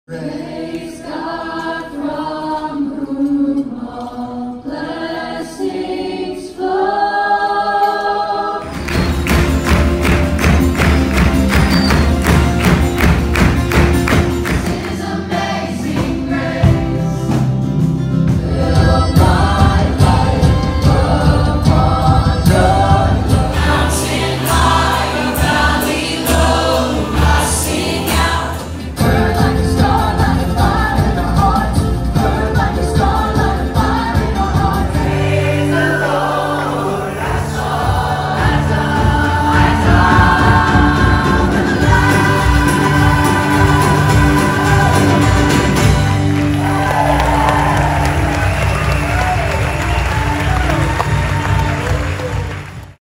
Called Higher Choir Demo
We are super stoked to be relaunching the Called Higher Community Youth Choir!  Our heart is to come together as the body of Christ and join as one voice in worship to our King. This is a community choir with youth from multiple churches.  The choir includes middle school, high school, and young adult age groups.